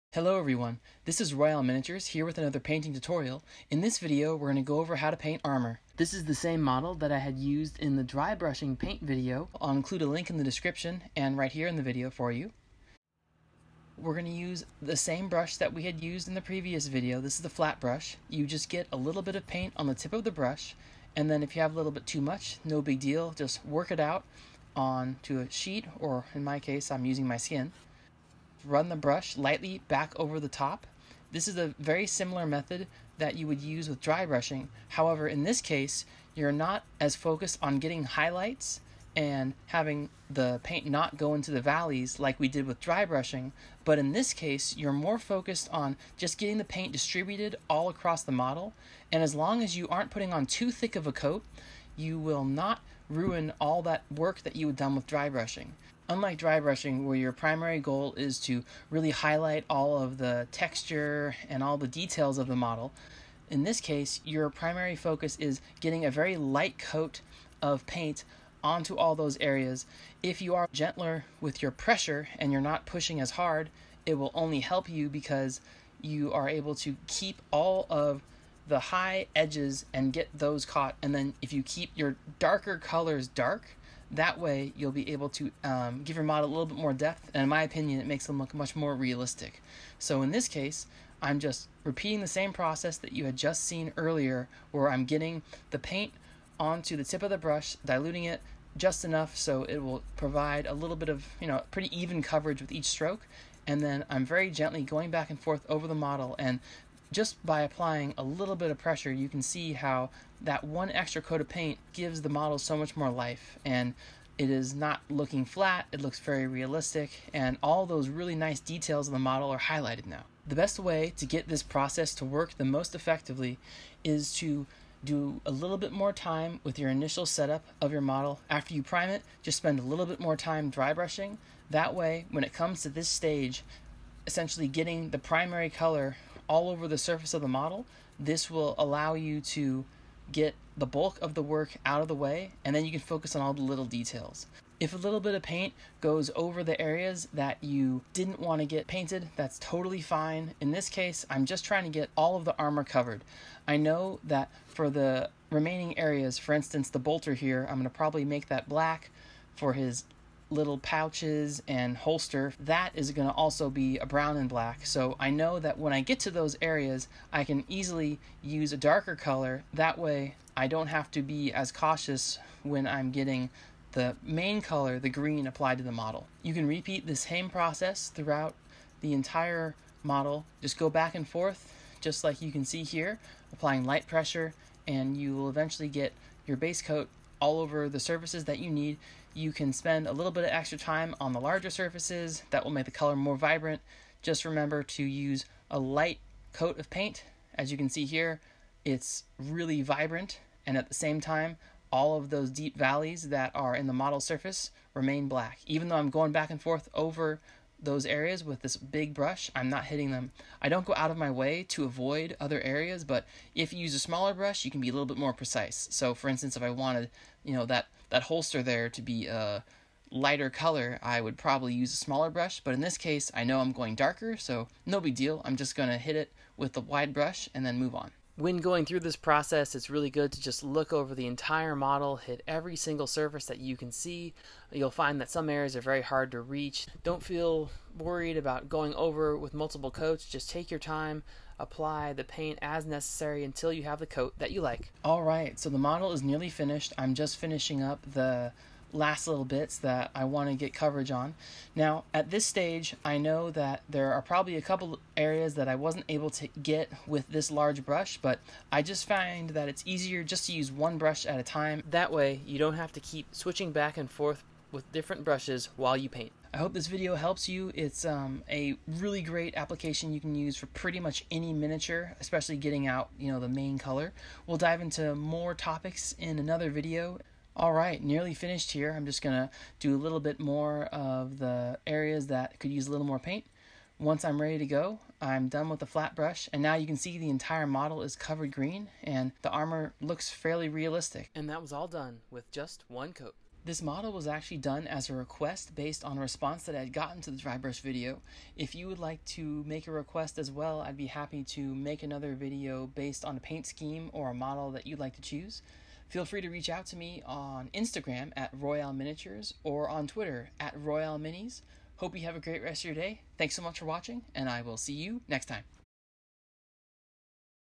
Painting Armor
Learn how to quickly get the bulk of your model painted using just a small amount of paint.  No air brush needed!  When painting the main color on your model it helps to knock it all out at once, I'll show you how to build a solid base for painting a miniature.

Hello Everyone!

This tutorial builds on the the 30 Second Dry Brush Video.  If you want to see how we got the model looking like this watch the link below on YouTube.

Now we focus on giving the model its main color.  This process is similar to dry brushing but much more forgiving.  I'll elaborate on this point as much as I can.

Hope this helps you get your models looking awesome!  Please enjoy.

-Royale Miniatures